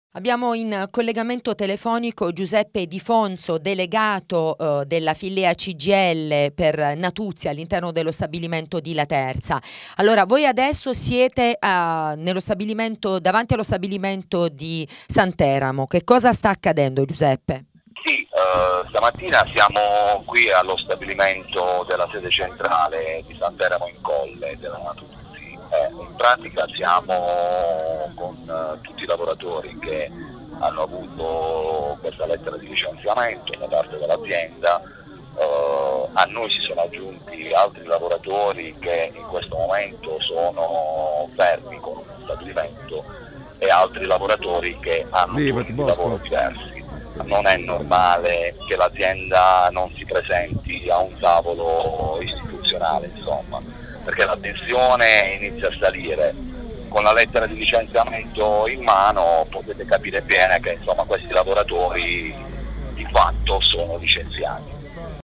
Natuzzi. I lavoratori presidiano lo stabilimento di Santeramo
Dopo la diserzione dell'azienda all'incontro di ieri in Regione, il clima attorno la vertenza Natuzzi si infiamma. In queste ore i lavoratori (quelli già raggiunti dalla lettera di licenziamento e gli altri in contratto di solidarietà), presidiano lo stabilimento principale di Santeramo in Colle.

Siamo quì per spiegare ai lavoratori cosa sta accadendo e cosa è accaduto ieri al tavolo dell'Assessorato al Lavoro della Regione Puglia - dice Giuseppe Difonzo, delegato della FILLEA CGIL - ma se l'Azienda dovesse ancora perseguire nel non dialogo credo che l'agitazione sarà inevitabile.

La cabina di regia per Natuzzi che ha già inviato circa 300 lettere di licenziamento tornerà a riunirsi il prossimo 21 ottobre sembre in Regione, nel frattempo proseguo le interlocuzioni tra il Gruppo e i vertici regionali con l'Assessore al lavoro Sebastiano Leo e il presidente della task force Leo Caroli.